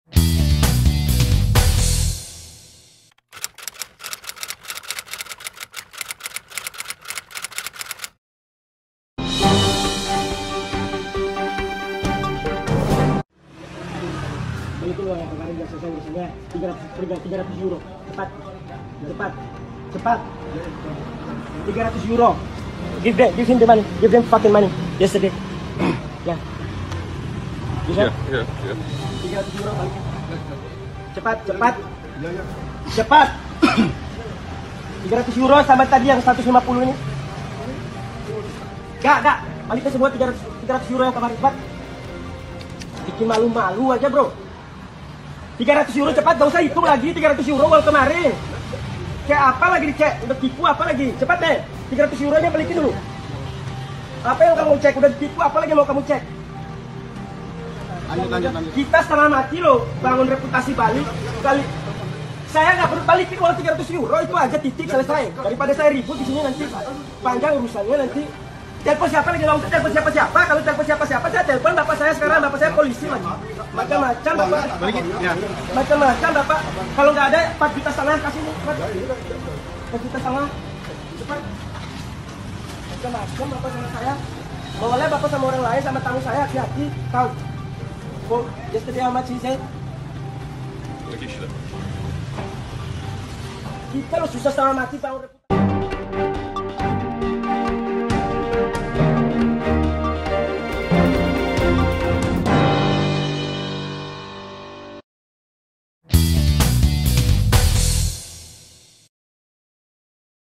Guide di Bali Labrak Money Changer Curang | duniadianFLASH
Viral video seorang guide di Bali yang melabrak money changer yang diduga curang dan diduga menipu turis asing tamunya.

Sang Guide tampak kesal dan memperingatkan agar jangan merusak citra dan reputasi pariwisata Bali dengan hal begitu.